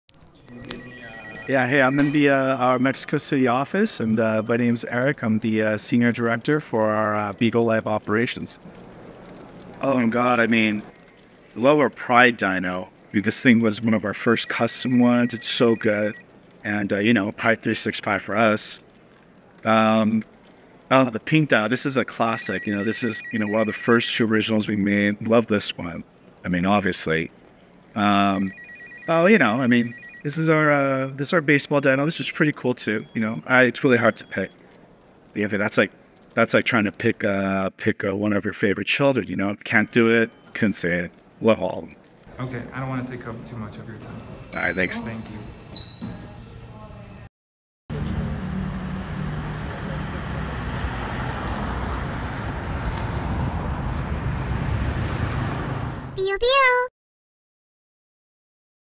The BIG(Office) - Eric's Favorite Dino
We had the opportunity to interview Eric during his visit to the CDMX Bigoffice. He really doesn't have a favorite Dino.